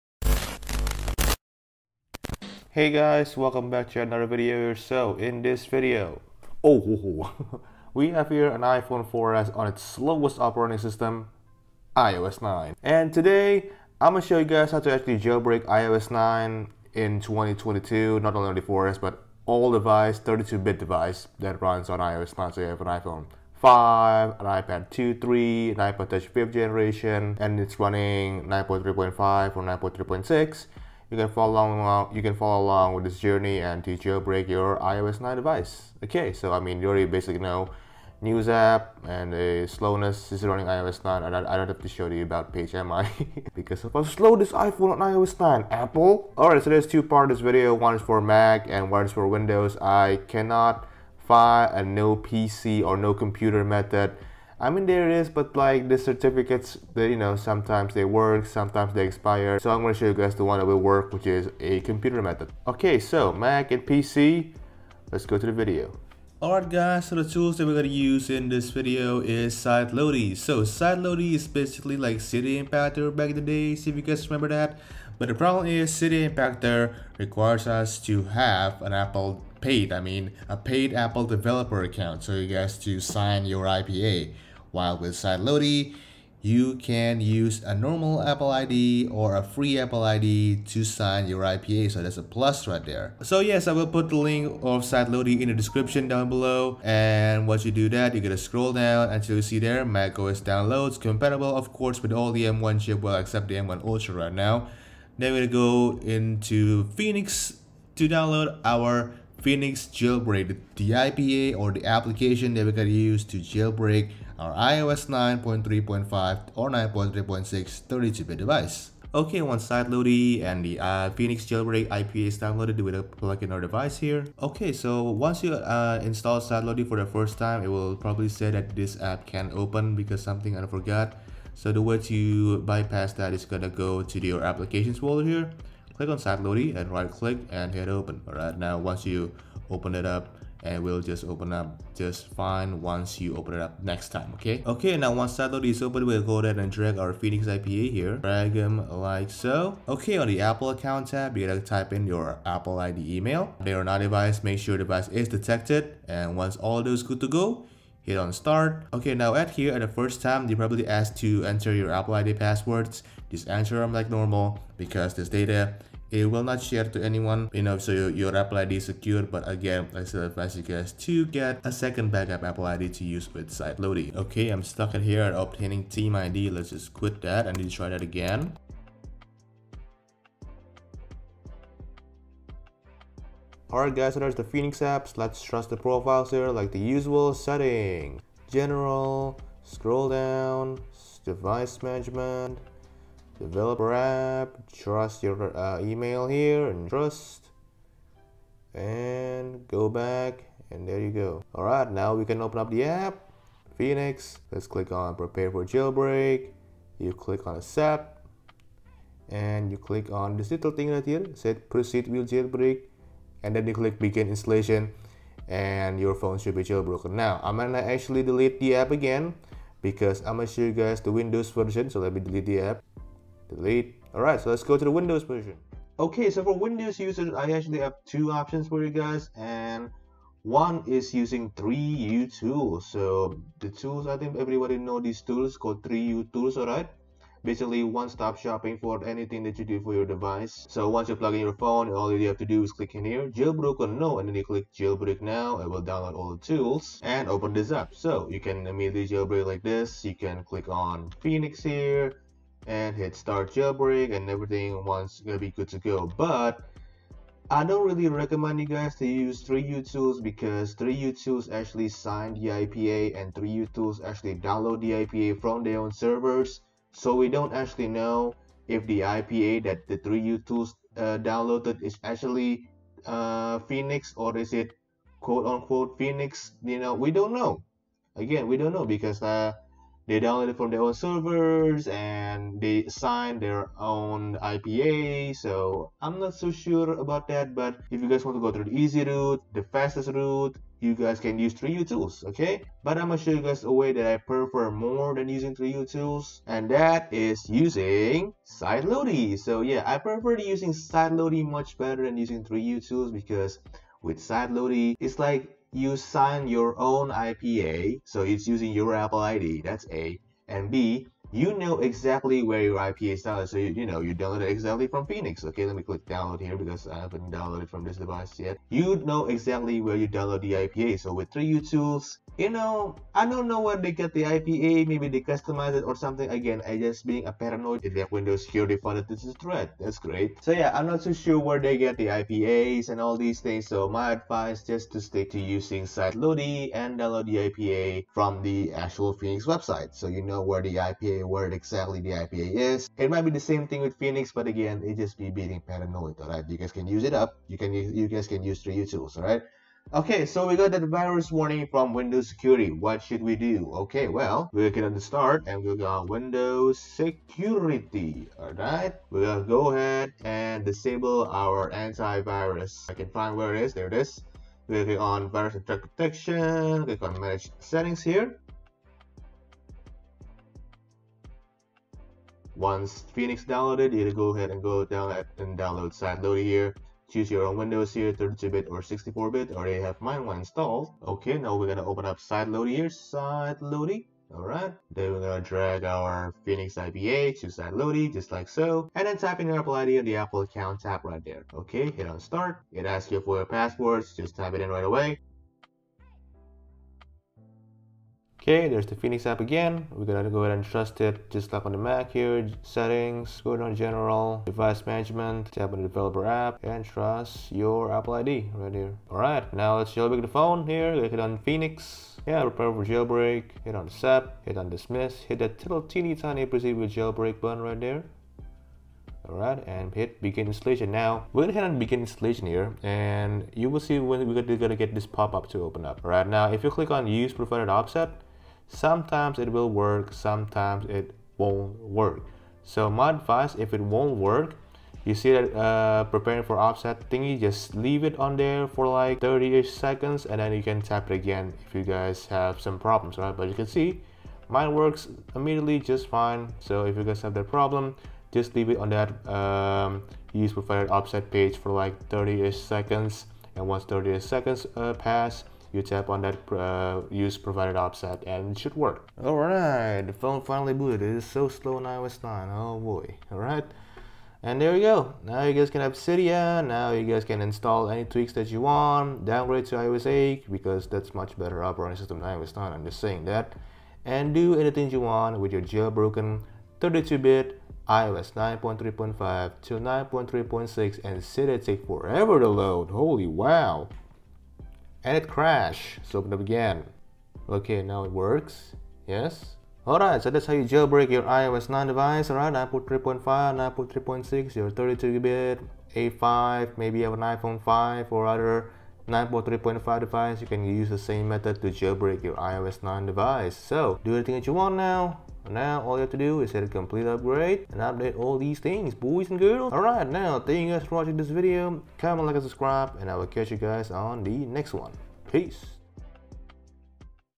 How to Jailbreak iOS 9.3.5 & 9.3.6 in 2024 (Mac & Windows)
In this video, we're going to find out on how to jailbreak iOS 9.3.5 & 9.3.6 on both Windows & Mac

00:00 intro
01:06 Mac Method
03:46 Windows Method
07:02 Quick tip if it's not working
07:40 Results